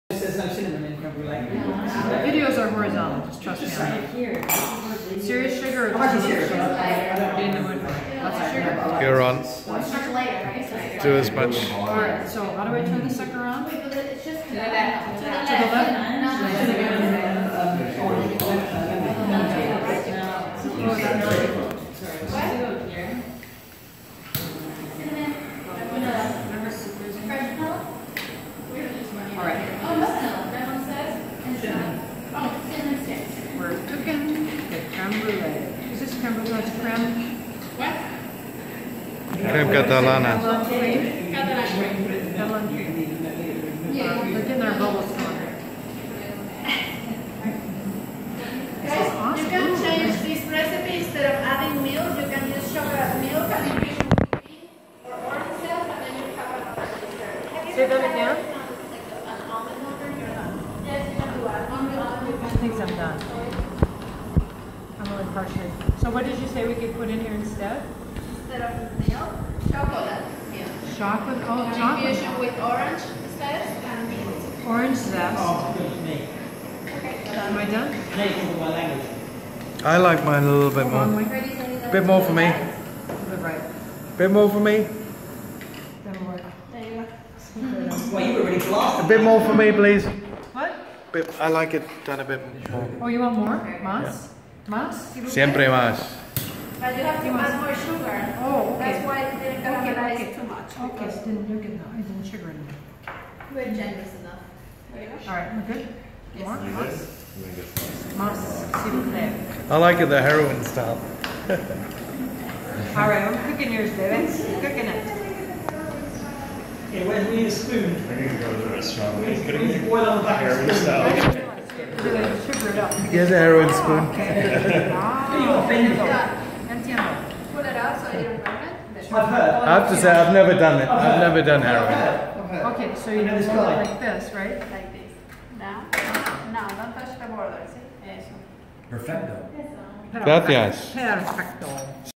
How to caramelize sugar using a torch on Catalan Crema, Spanish creme, creme brulee
Cooking class at Barcelona Cooking. We made Crema Catalan  and I learned how to caramelize the sugar topping using a kitchen torch. What's more fun than lighting food on fire?!